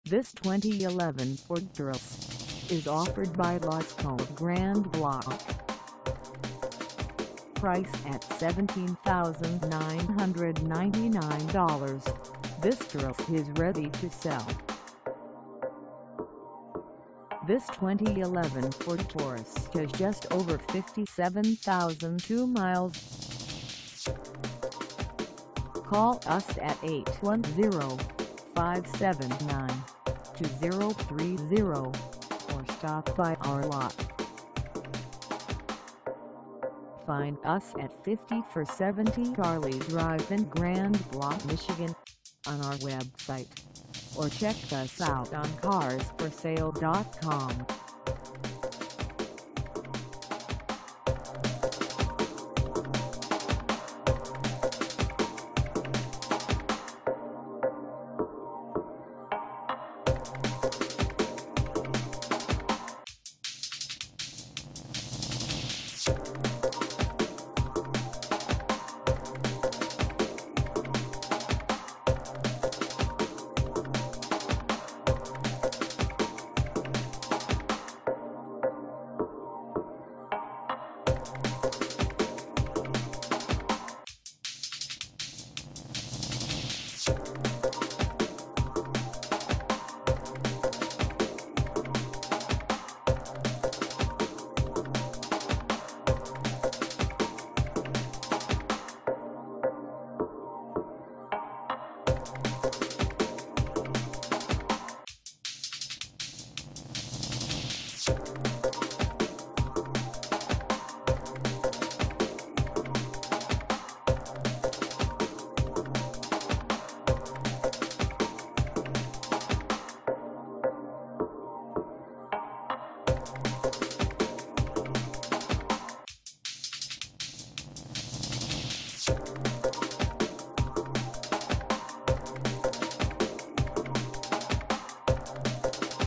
2011 Ford Taurus SE 4dr Sedan for sale in Grand Blanc, MI 48
This 2011 Ford Taurus SE 4dr Sedan is for sale in Grand Blanc, MI 48439 at Lasco of Grand Blanc.
Engine: 6-Cylinder 3.5L
Transmission: Automatic 6-Speed
Color: Unspecified
Mileage: 57002
Contact Lasco of Grand Blanc 5470 Ali Dr Grand Blanc, MI 48439 to test drive this 2011 Ford Taurus SE 4dr Sedan today.